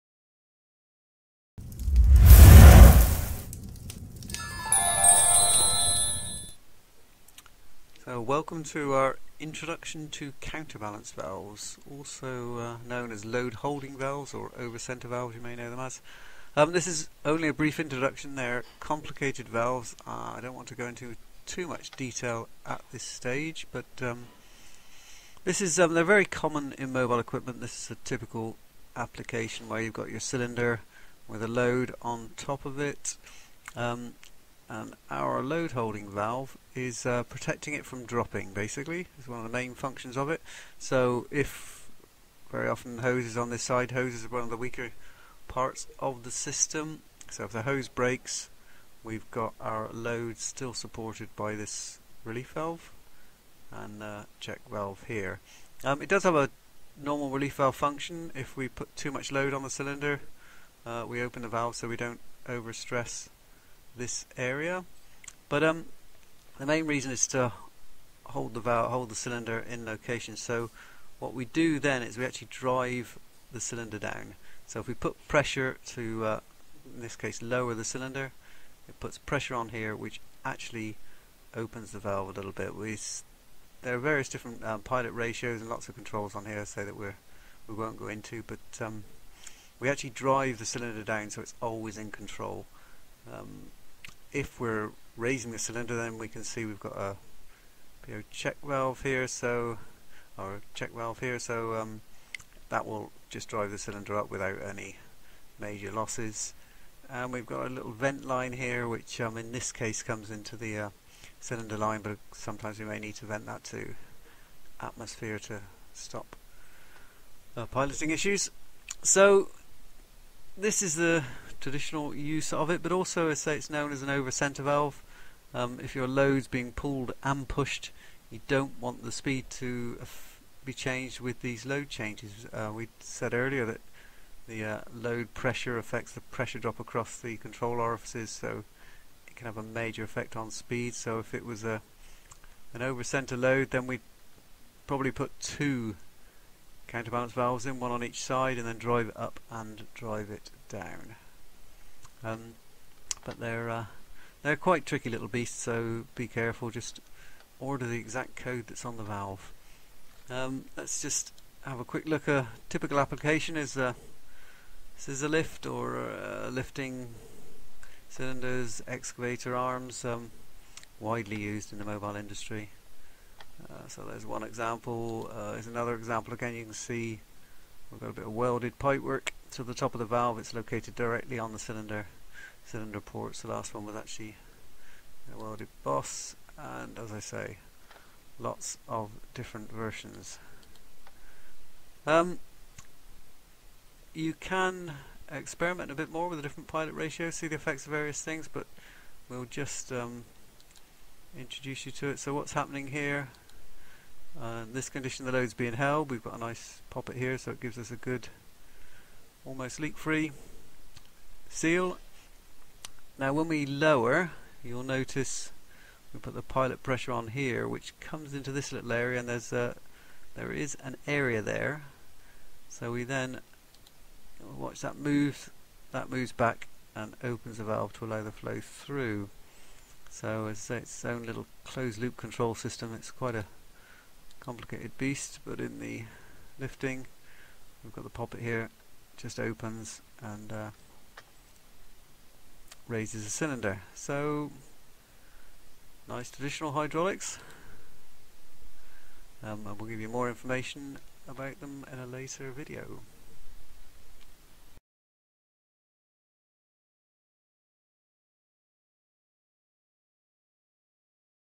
Hydraulic counterbalance, load control and valve operation, uses and types HV15
Learn how hydraulic counterbalance valves work. Understand the different types of valve that are available, their function and where they are used.